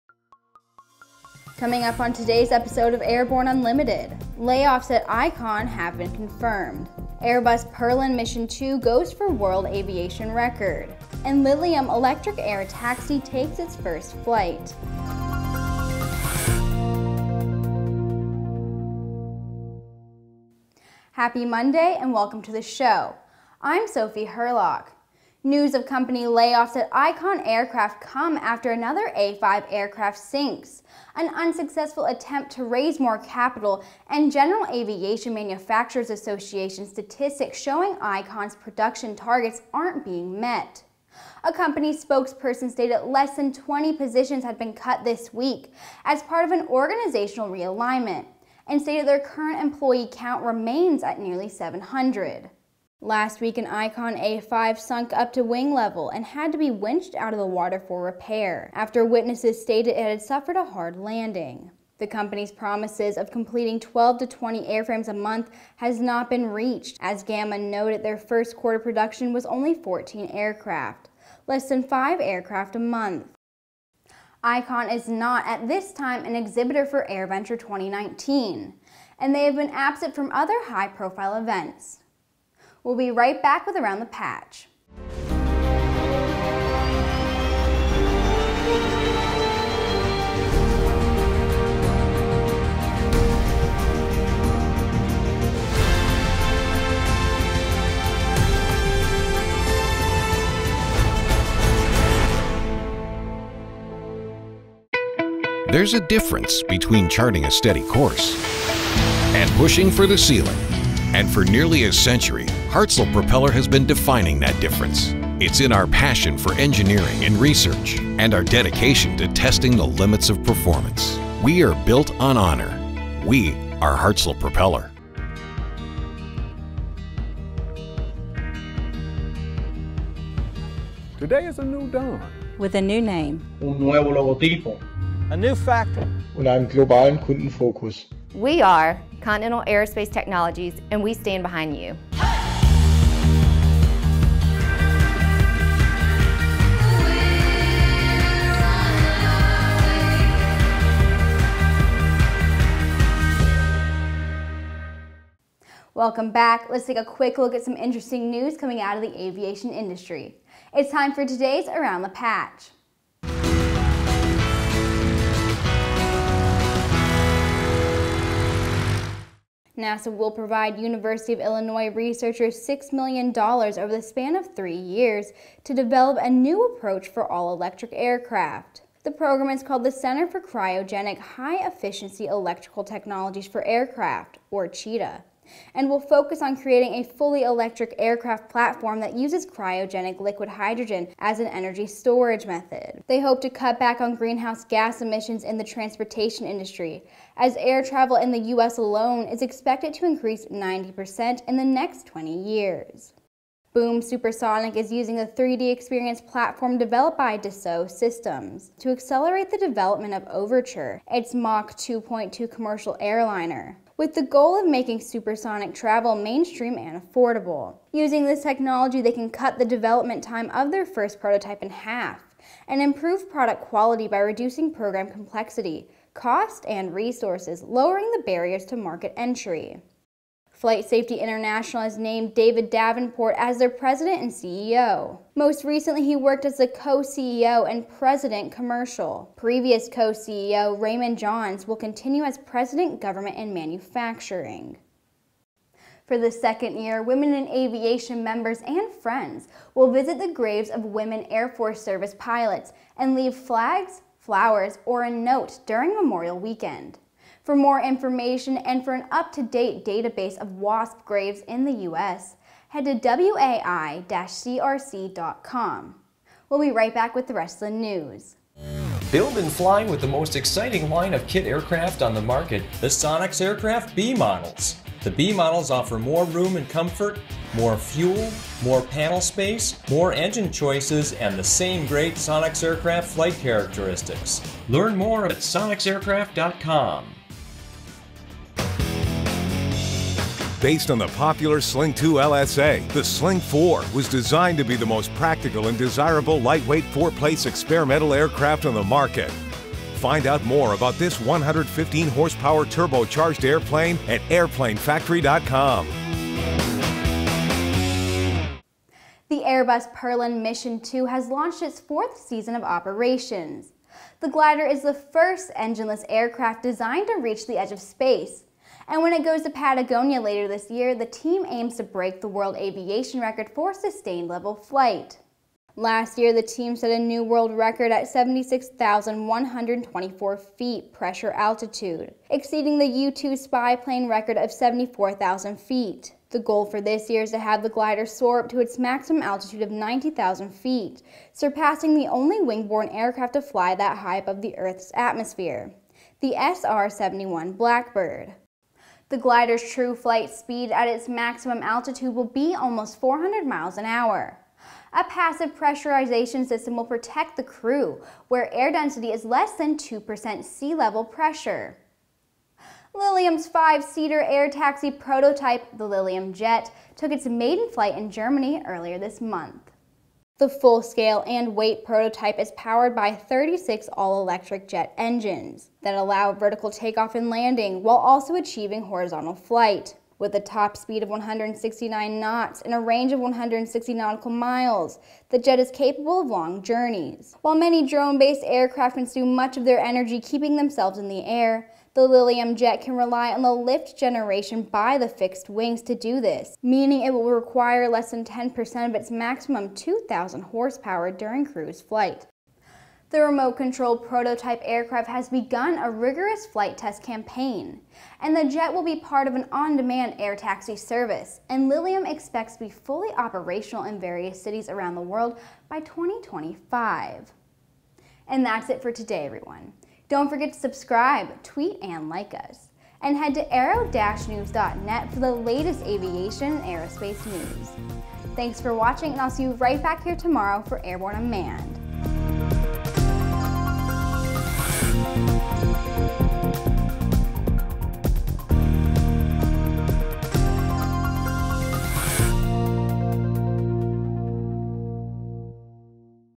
Airborne 05.20.19: Icon Layoffs, Perlan Mission II, Lilium Air Taxi
Also: HonorTheWASP, NASA Funds Research, Boom Supersonic And Dassault, New FlightSafety CEO

Amid recent reports of yet another sinking of an Icon A5 airframe, a heretofore unsuccessful attempt to raise more capital, and GAMA stats showing that Icon’s promised production targets are not being met, along comes news of company layoffs. An Icon A5 was sunk in Washington state last week after witnesses reported it to have suffered a hard landing... not the first such report. The airframe sunk to wing level and required winching out of the water, for repair. Reported attempts to raise additional capital appear to be unproductive and promises of completing as many as 12-20 airframes per month have definitely not been reached as GAMA noted that their First Quarter production total tallied only 14 aircraft... less than 5 birds per month. Airbus Perlan Mission II has launched its fourth season of operations. The Perlan 2 glider is the world’s first engineless aircraft designed to reach the edge of space. When the aircraft deploys to Patagonia later this year, the team aims to achieve the world altitude aviation record for sustained, level flight. Last year, on Sept. 2, 2018, the team set a new world record of 76,124 feet pressure altitude, surpassing the highest known flight of the U-2 (74,000 feet). Lilium has taken the wraps off its new five-seater air taxi prototype. The Lilium Jet completed its maiden flight in Germany earlier this month. The full-scale, full-weight prototype is powered by 36 all-electric jet engines that allow it to take-off and land vertically, while achieving horizontal, or cruise, flight. With a top speed of 160 knots and a range of 160 nautical miles, the Lilium Jet is reportedly capable of completing long journeys. All this -- and MORE in today's episode of Airborne!!!

Airborne 05.20.19 is chock full of info in this Daily News Episode, Monday, May 20th, 2019... Presented by Aero-TV veteran videographer and Airborne Hosts Sophie Hurlock, Laura Hutson, Brianne Cross, and Christopher Odom, and is supported by ANN CEO/Editor-In-Chief Jim Campbell, Chief Videographer Nathan Cremisino, and Aero-Journalist Tom Patton.